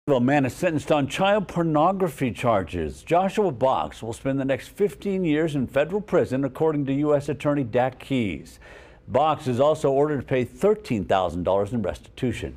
Fayetteville man sentenced to 15 years in prison on child porn charges
Joshua Box pleaded guilty in August 2018.